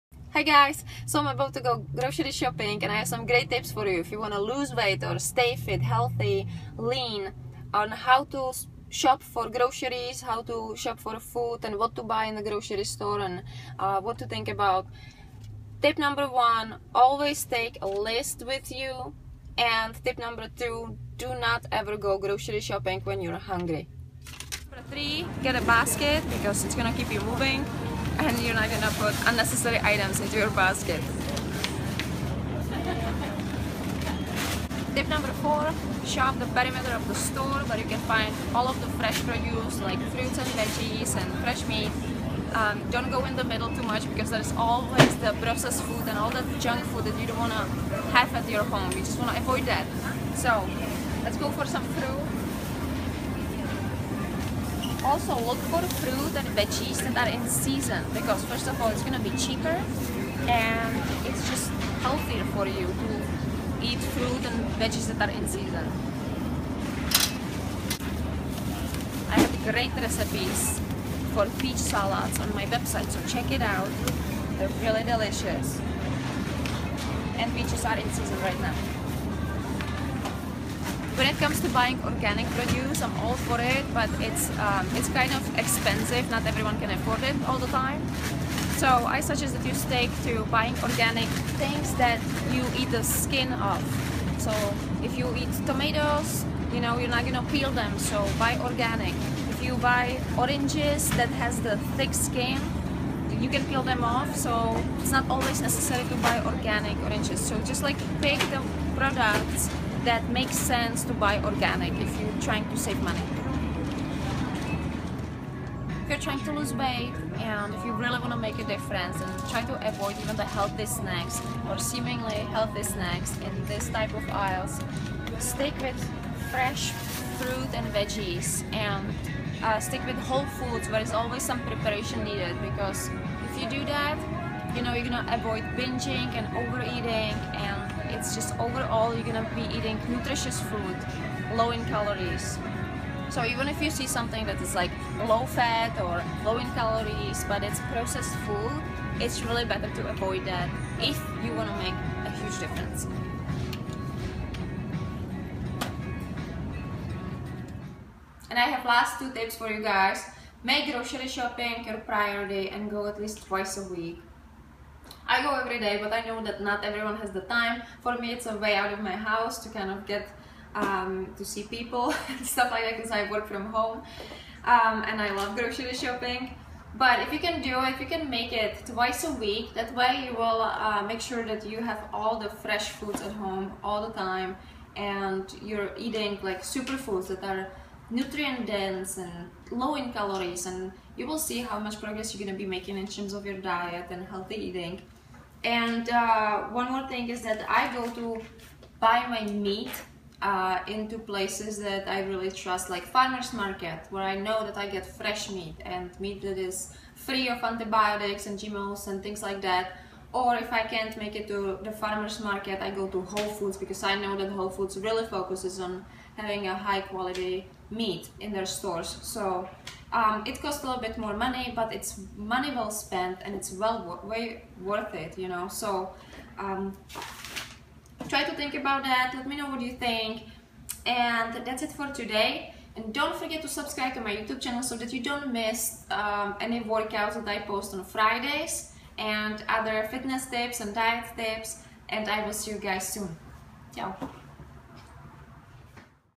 Grocery Shopping for Weightloss
If your goal is to lose weight or get leaner and lose fat, these tips for grocery shopping will help you to make better and healthier choices. Here's the full blog post